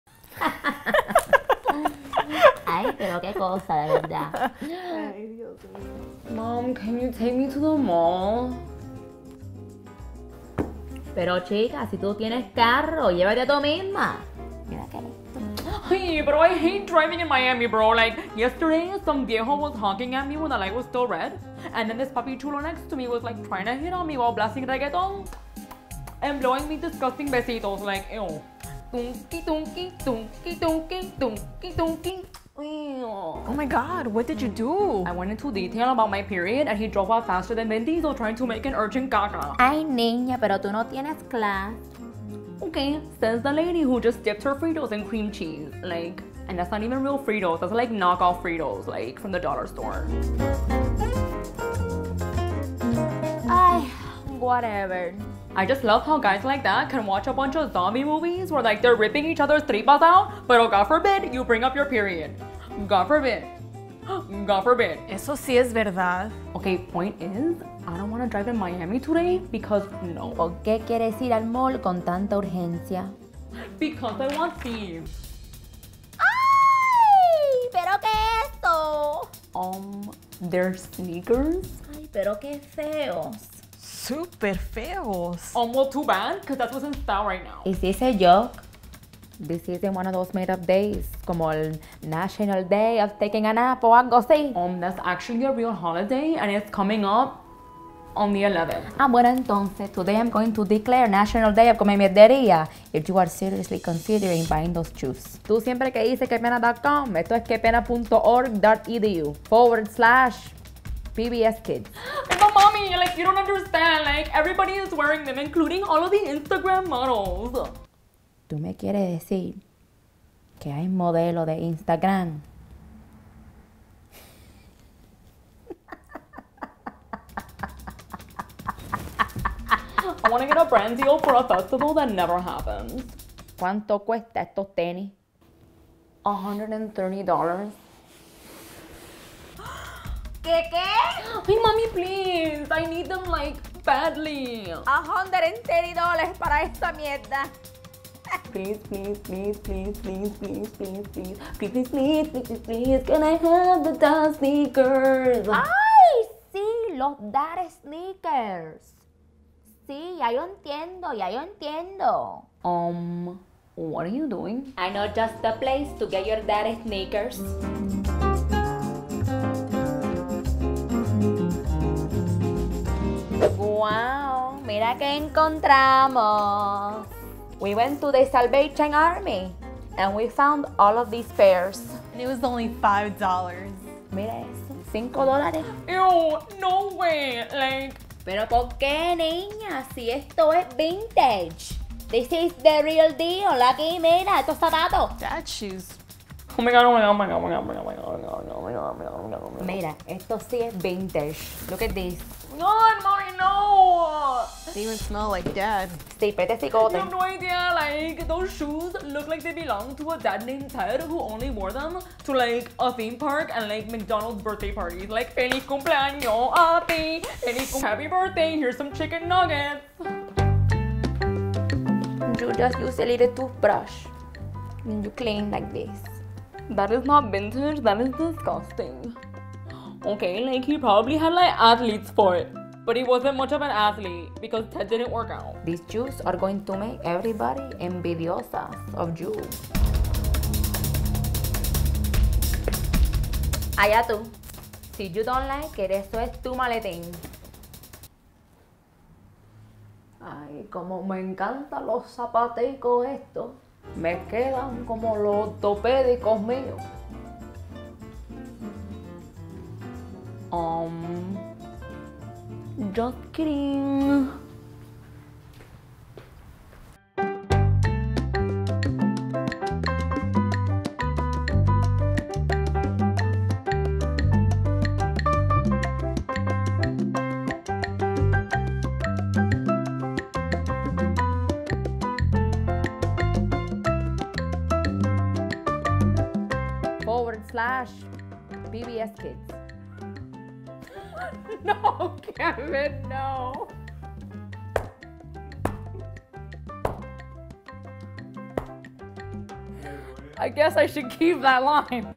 LATINA MOM reacts to ugly DAD SNEAKERS
Everyone's going crazy over Dad Sneakers, including Laritza. Pero, who could blame her for wanting to be en la moda? Even though this is clearly a trend that's gonna die, like, tomorrow. Quefeo [dot] com. 👟👟👟

Where's Jenny, meng?

[CREW]

Addt'l Writing: Kevin Bosch

[CAST]